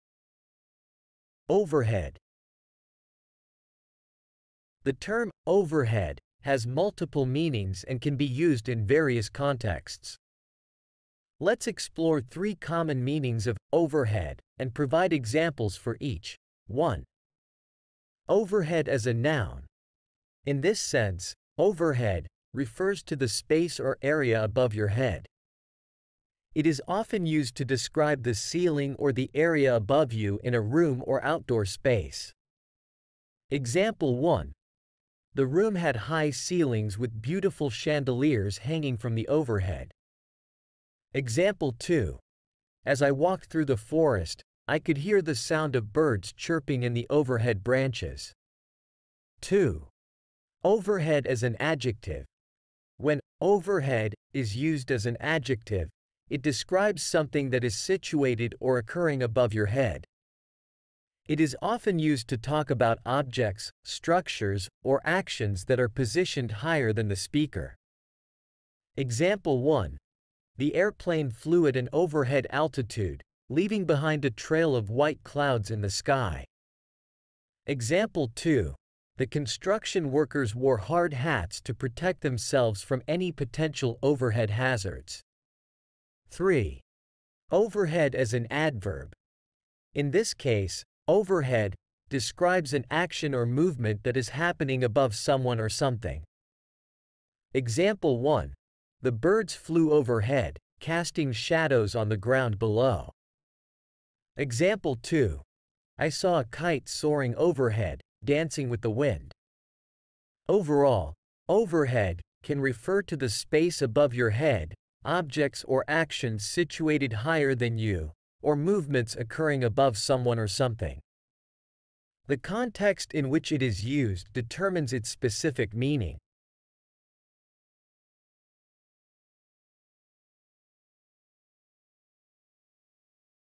Overhead
The term "overhead" has multiple meanings and can be used in various contexts. Let's explore three common meanings of "overhead" and provide examples for each:
1. Overhead as a noun:
In this sense, "overhead" refers to the space or area above your head. It is often used to describe the ceiling or the area above you in a room or outdoor space.
Example 1:
The room had high ceilings with beautiful chandeliers hanging from the overhead.
Example 2:
As I walked through the forest, I could hear the sound of birds chirping in the overhead branches.
2. Overhead as an adjective:
When "overhead" is used as an adjective, it describes something that is situated or occurring above your head. It is often used to talk about objects, structures, or actions that are positioned higher than the speaker.
Example 1:
The airplane flew at an overhead altitude, leaving behind a trail of white clouds in the sky.
Example 2:
The construction workers wore hard hats to protect themselves from any potential overhead hazards.
3. Overhead as an adverb:
In this case, "overhead" describes an action or movement that is happening above someone or something.
Example 1:
The birds flew overhead, casting shadows on the ground below.
Example 2:
I saw a kite soaring overhead, dancing with the wind.
Overall, "overhead" can refer to the space above your head, objects or actions situated higher than you, or movements occurring above someone or something. The context in which it is used determines its specific meaning.